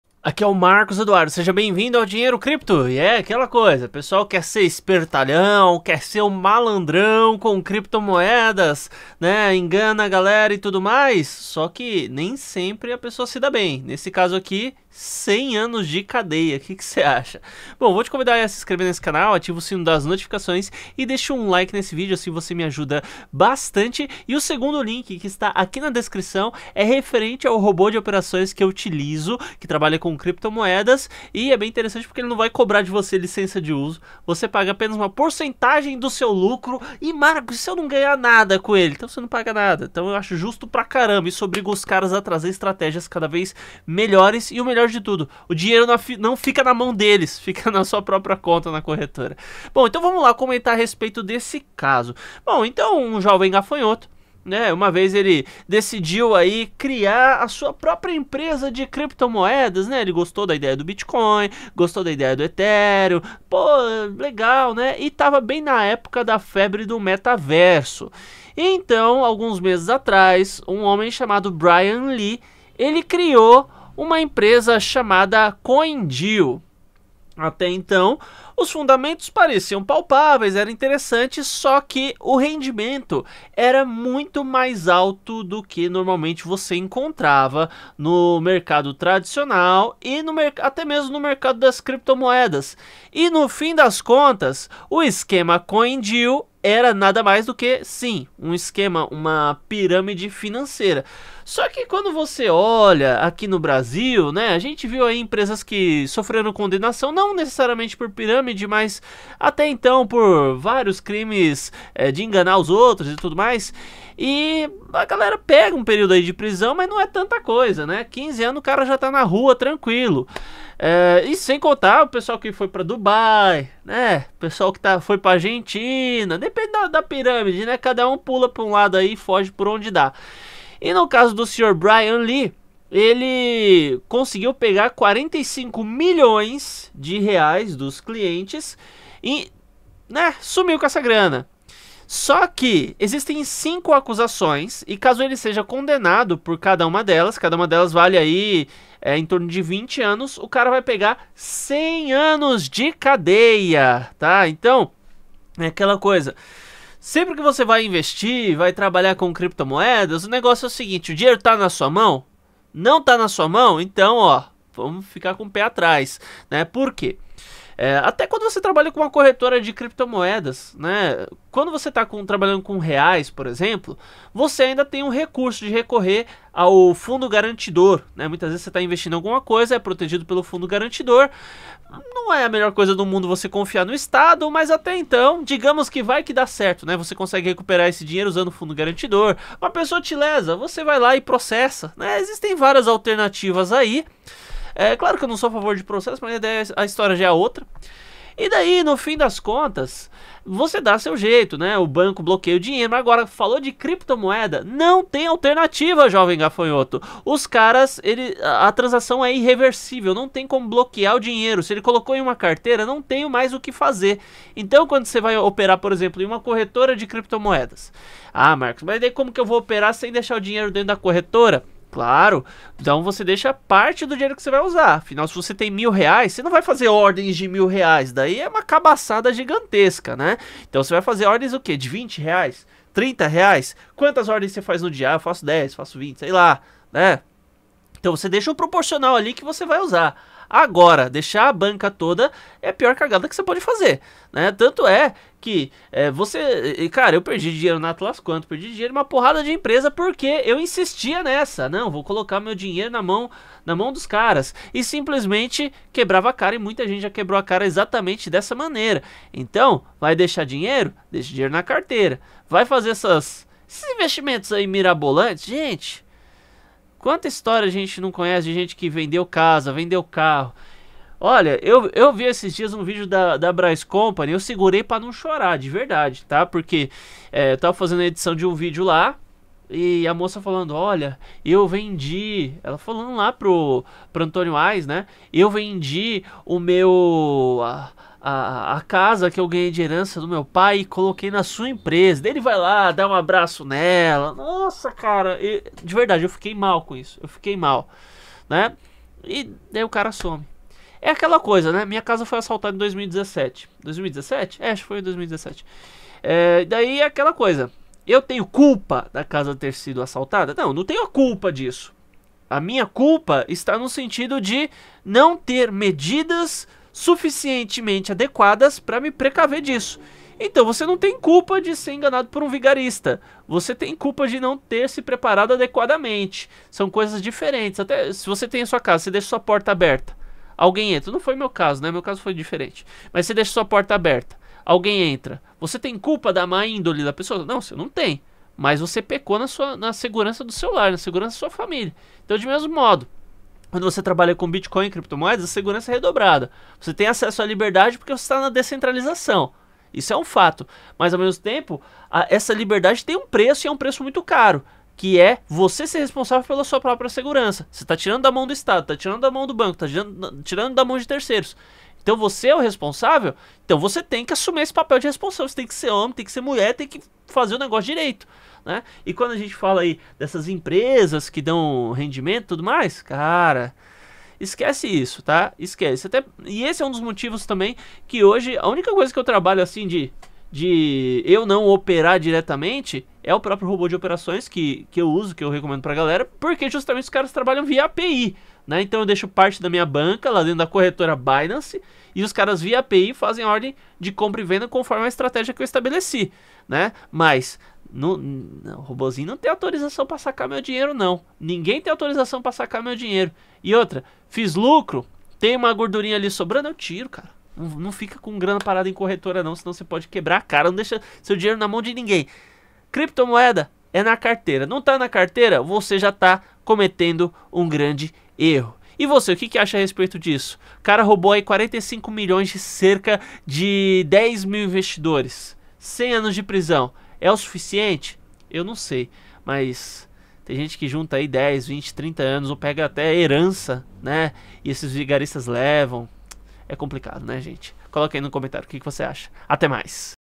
🔥Esse homem pode pegar 100 ANOS DE PRISÃO pela forma como usou as CRIPTOMOEDAS! 💰⚖️
Entenda como um homem conseguiu fraudar milhares de investidores em criptomoedas, levando $45 milhões para um destino ilícito. Saiba como se prevenir de esquemas de investimento fraudulentos no mundo blockchain e proteger seu dinheiro. Não caia na promessa de retornos "extremamente altos" sem garantia. Neste vídeo, descubra a história por trás da fraude do CoinDeal e aprenda a identificar sinais de alerta.